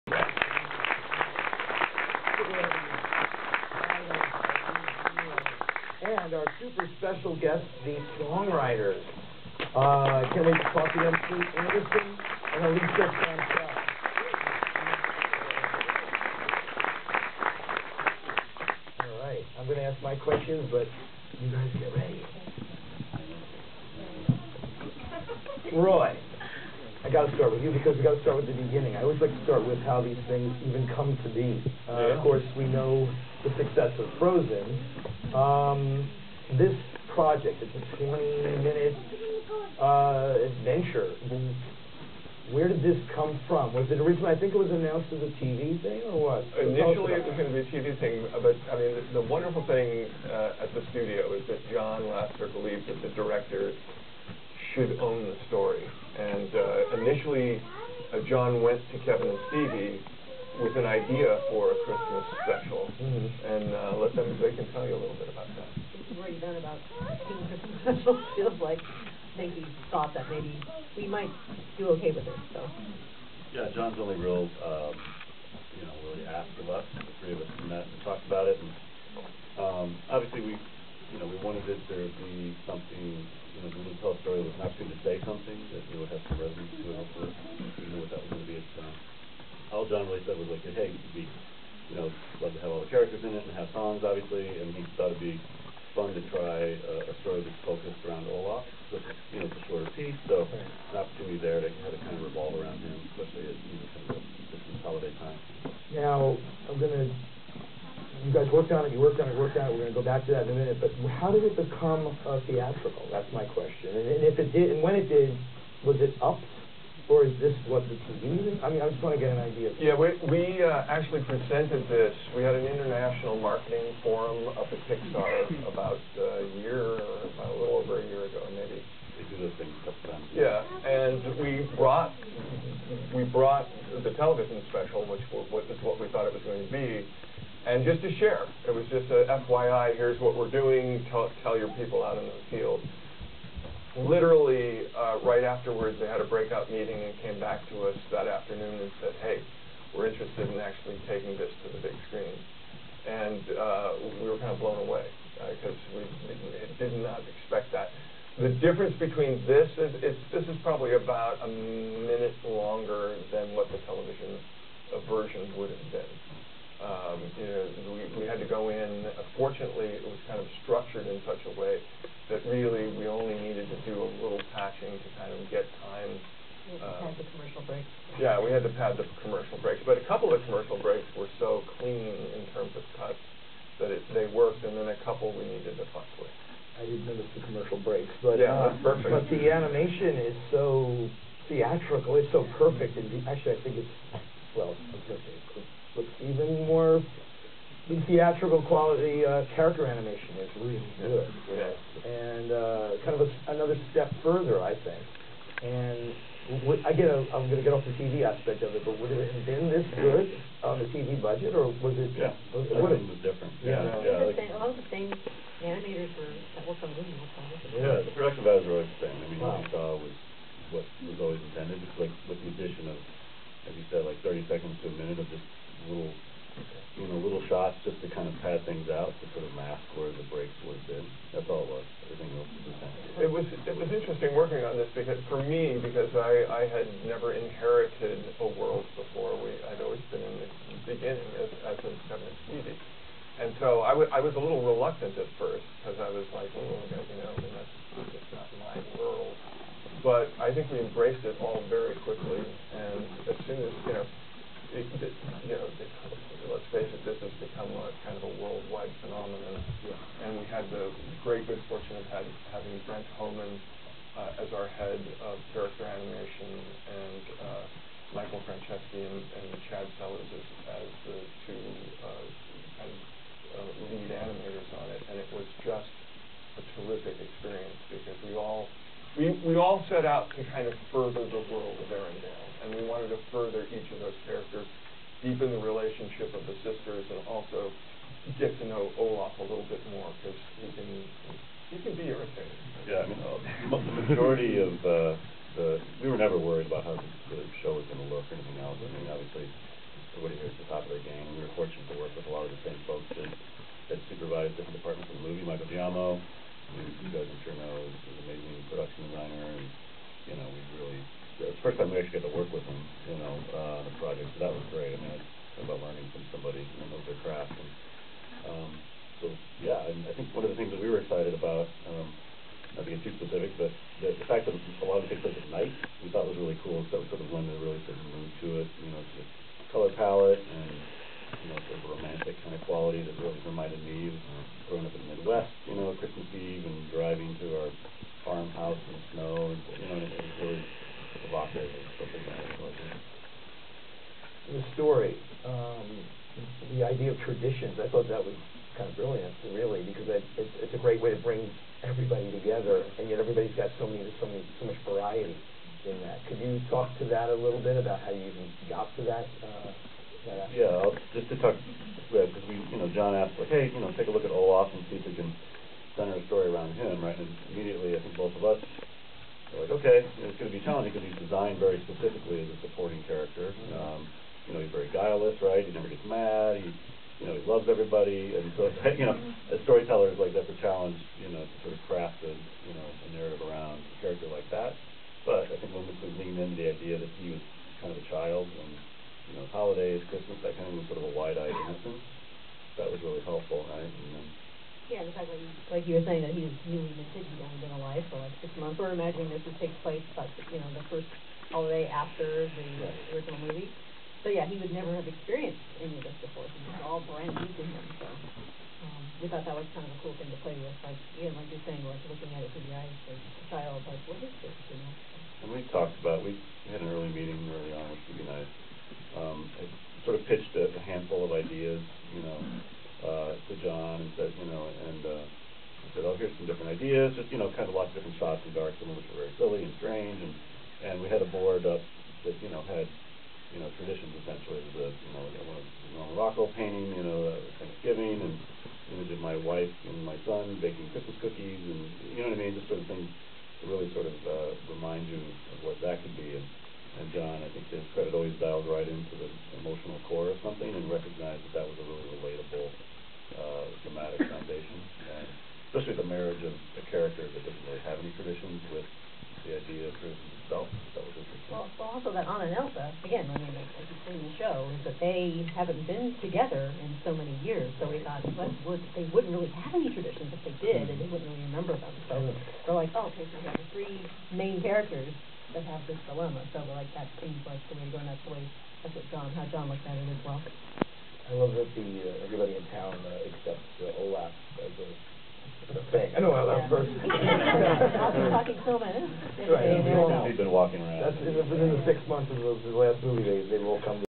Olaf's Frozen Adventure with Jerry Beck at Disney.
ASIFA's Jerry Beck, on stage, with the Disney creatives of Olaf's Frozen Adventure and the composers visiting from New York, at Walt Disney Animation. Stevie Wermers-Skelton, Kevin Deters, Roy Conli, and composers Elyssa Samsel and Kate Anderson.